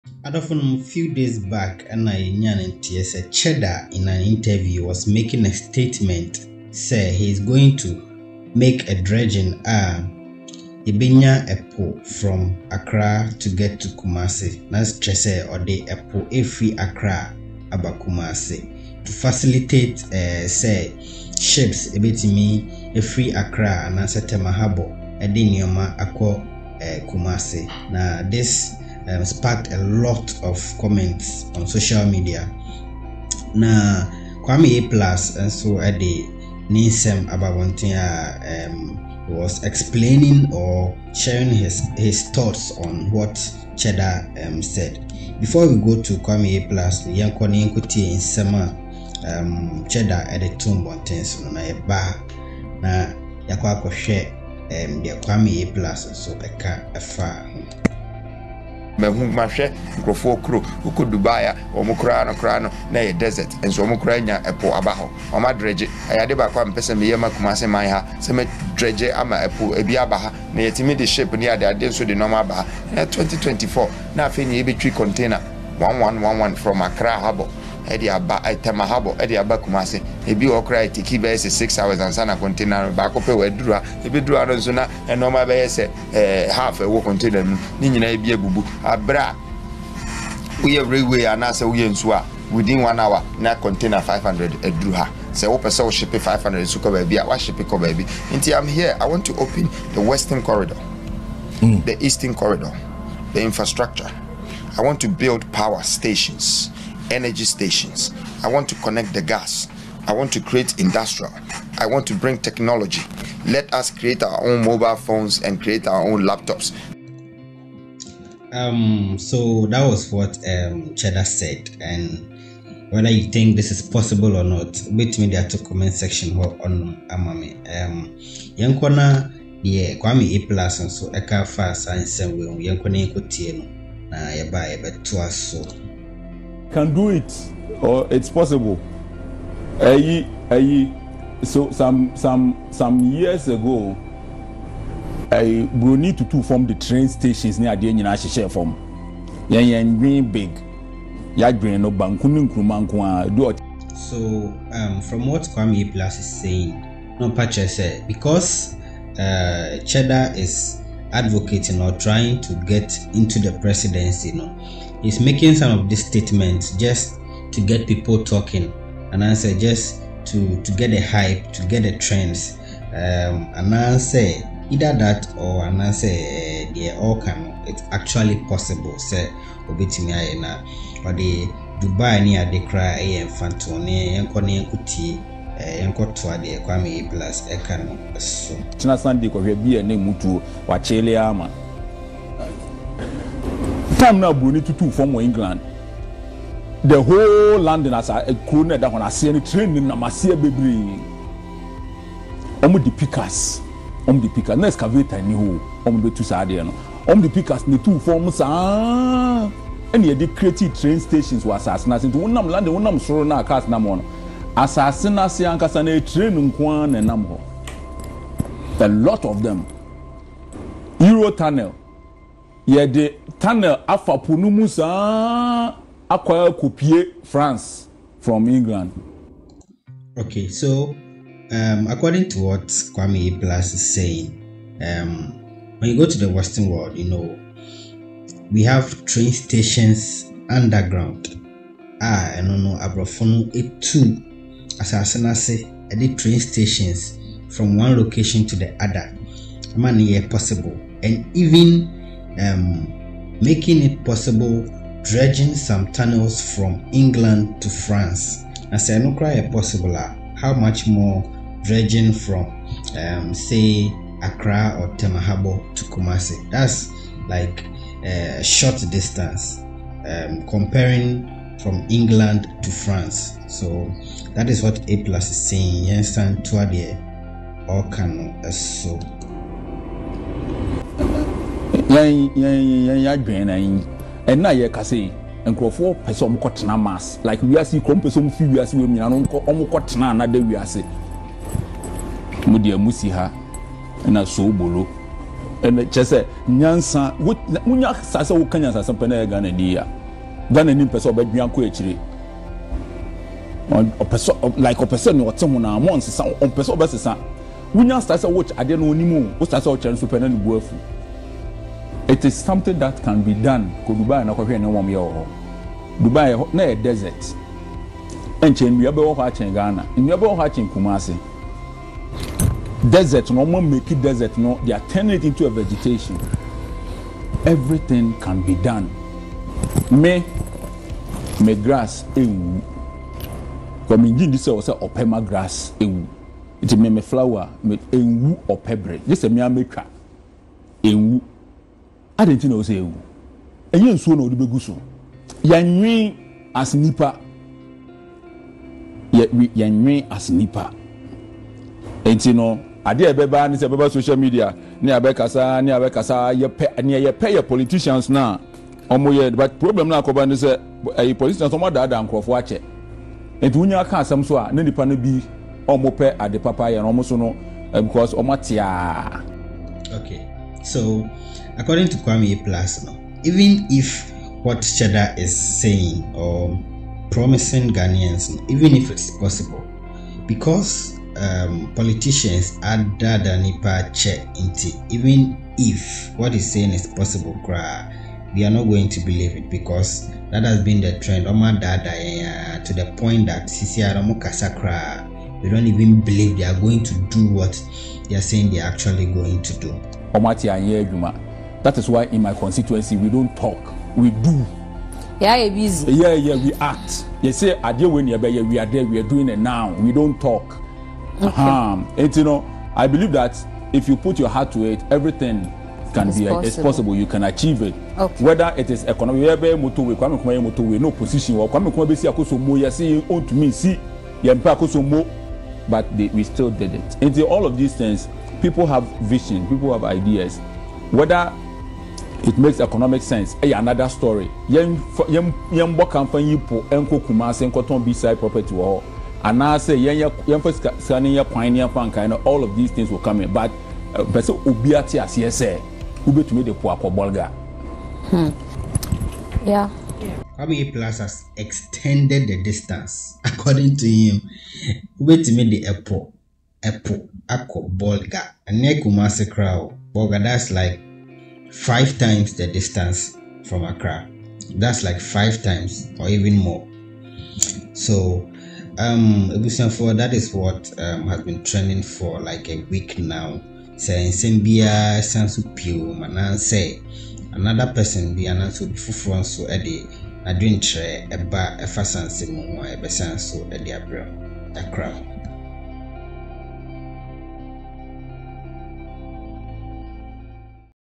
A-Plus Explains Cheddar's Promise to Dredge The Sea from Tema Habour to Kumasi - DON'T SKIP
Join us in this captivating episode as A-Plus delves deep into Cheddar's Promise, a groundbreaking initiative set to transform the landscape of Ghana. Witness the monumental journey of dredging the sea from Tema Harbour to Kumasi, unlocking new opportunities and reshaping the future of transportation and commerce. With expert insights and stunning visuals, discover how this ambitious project is poised to revolutionize regional connectivity and propel economic growth.